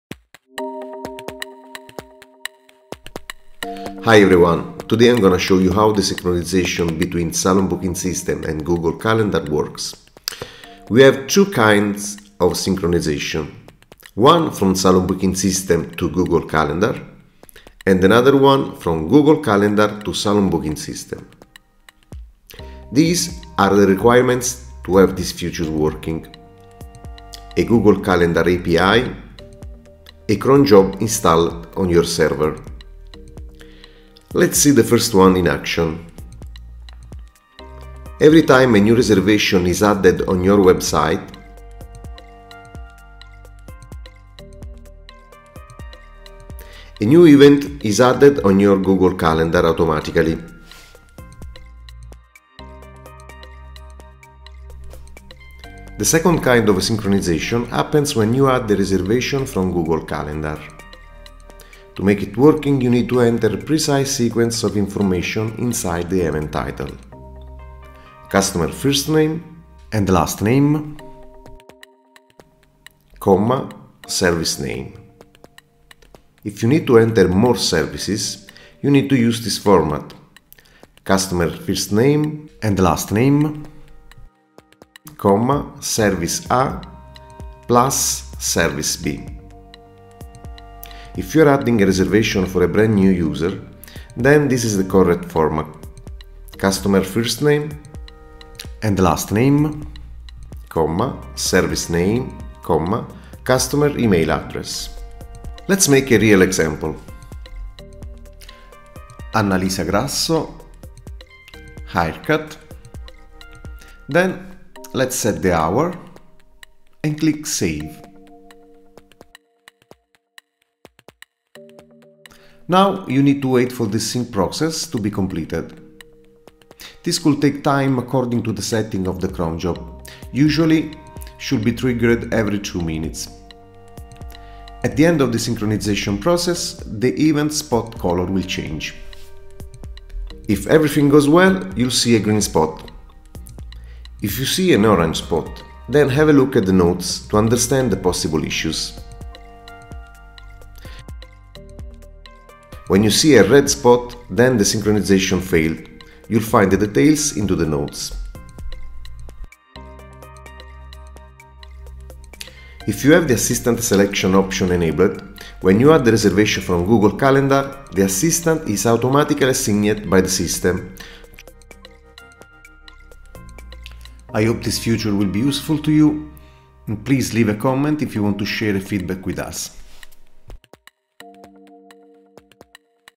Salon Booking System / Google Calendar synchronization
In this video, I'll explain how the synchronization between Salon Booking System and Google Calendar works.

For more details on how to create the Google Calendar API and how to set the cronjob, please visit this page: